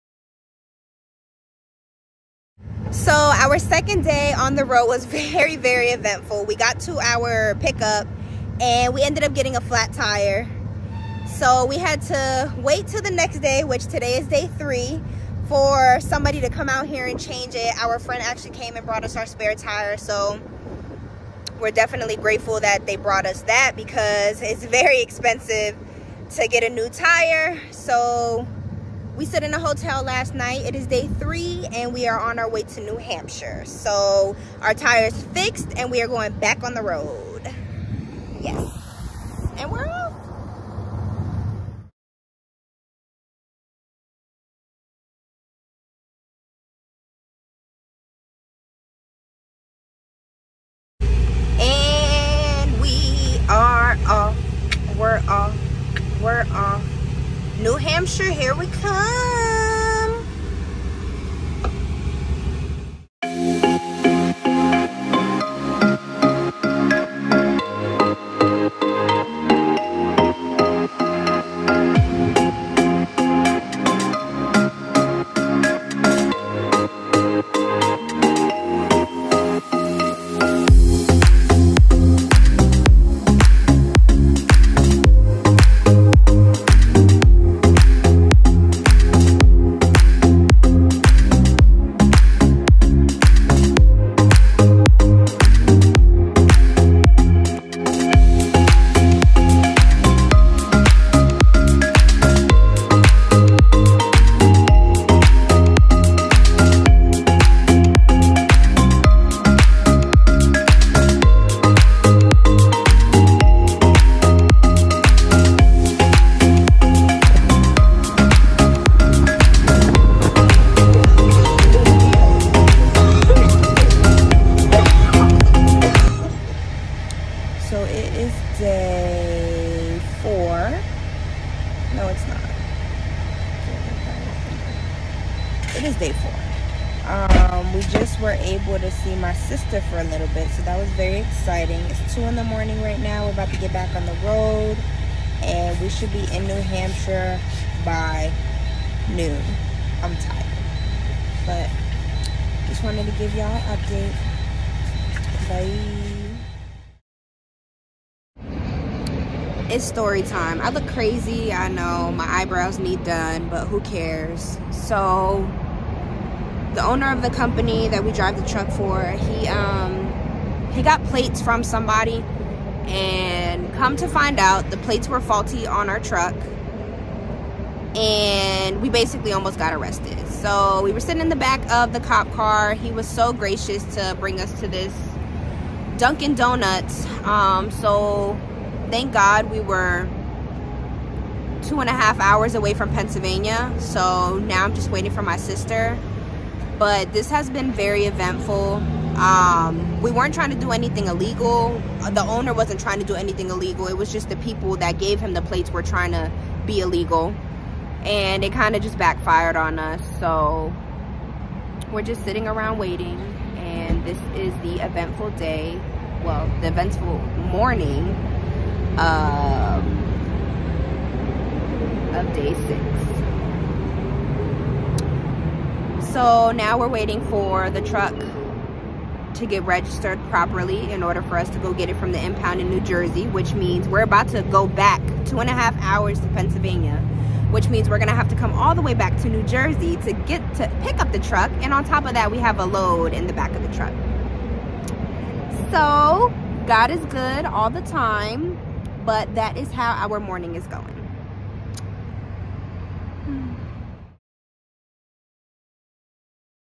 On the road mini vlog… POLICE GOT US!
Me and my husband were on the road for a week together. It was a crazy adventure, but our crazy moments blessed others in the process. I am extremely grateful and wouldn't change anything about it.  I hope you enjoy this mini vlog.